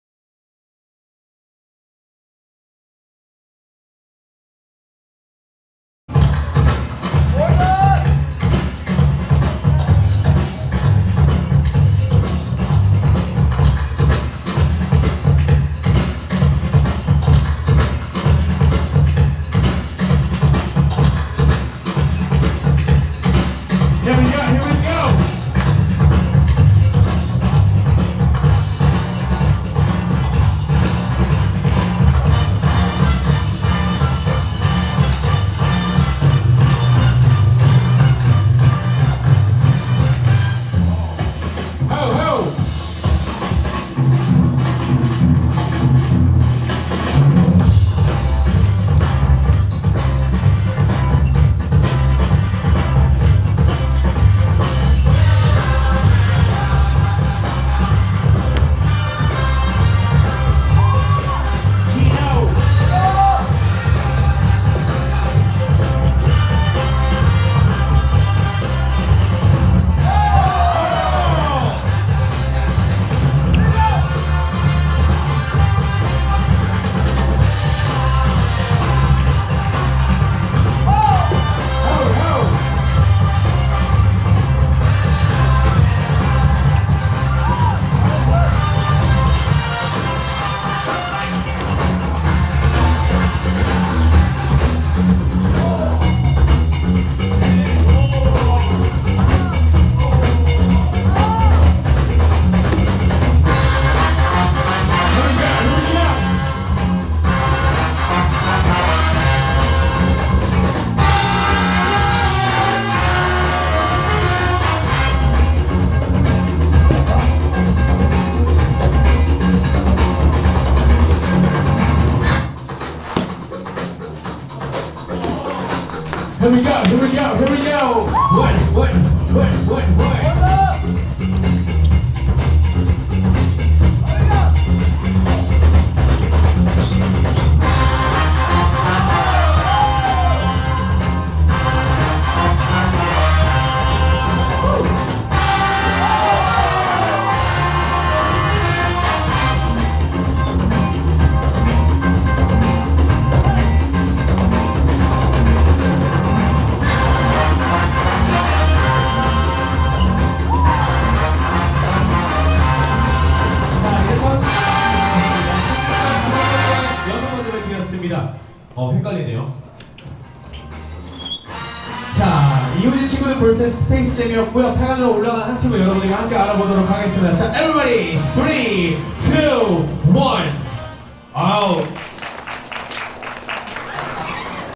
Top16 | Leewoojin & Friend VS Space Jam | Leadmos 10th Year Anniversary | LB-PIX
- Leadmos Bboy Session
10th Year Anniversary -

일시:2017년10월22일 일요일 오후2시
장소:DU Play Ground (서울특별시 관악구 신림동 1655-42 지하1층)

-MC-
 RP

-DJ-
 Moglv

-JUDGE-
 J-Hawk (T.G Breakers)
 Octopus (Jinjo Crew)
 Madmon (Fusion MC)

-SHOWCASE-
 Leadmos Fam
 Questribe

master film&drone film
*LB PIX SNS

highlight film
*Stance SNS

drector&motiongraphic&backstage film
*ChanPD SNS